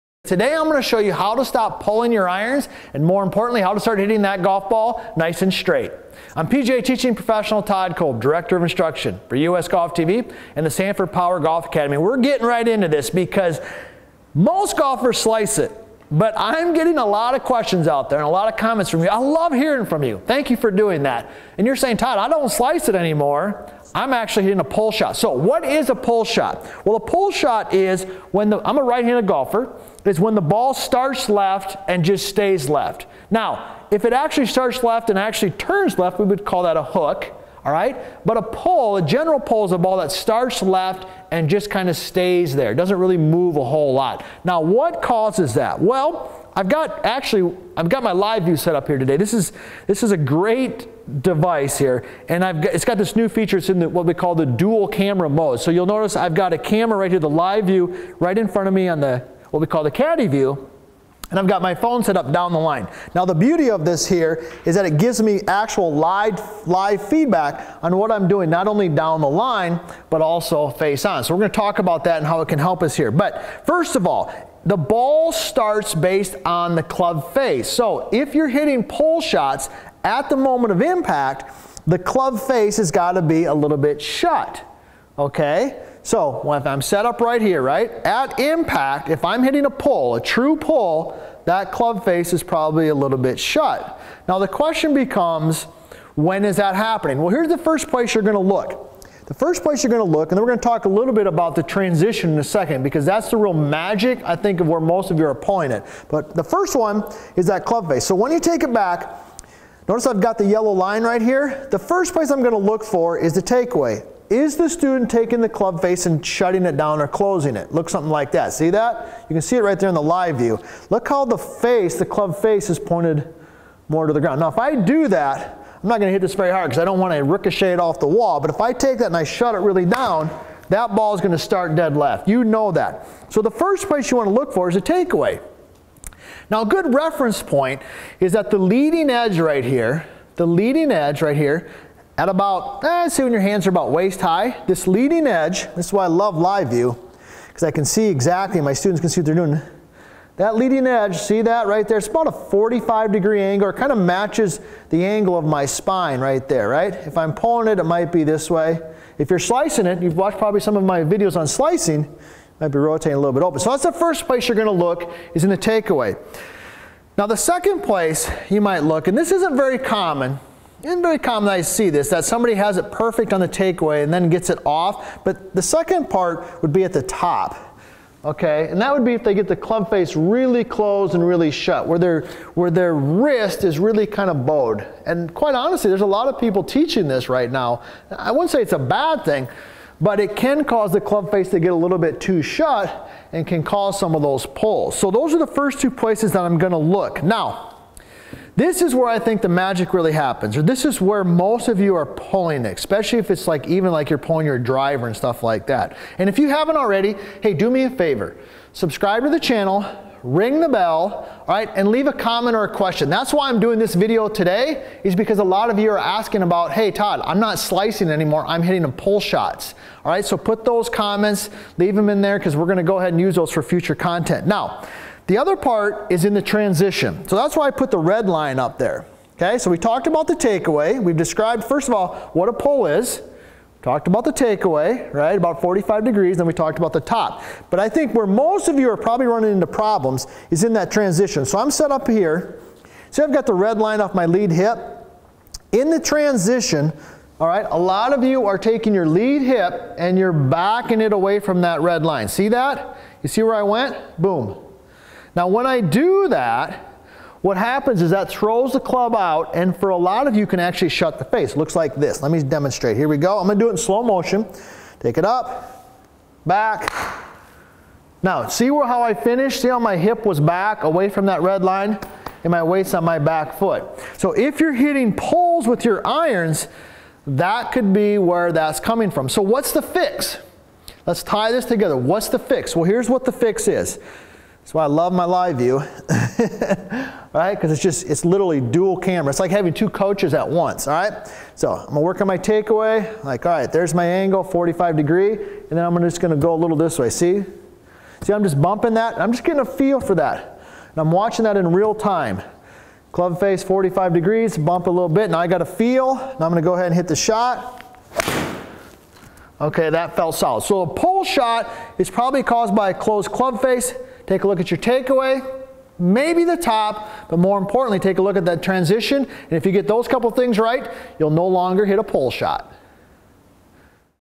How to Stop Pulling Iron Shots
Get $40 off a Live View Golf camera

In this golf lesson, we are going to show you how to stop pulling iron shots by showing you a few reasons why you hit a pull shot, using our Live View Pro to demonstrate the faults and fixes.  Stop pulling golf shots in no time!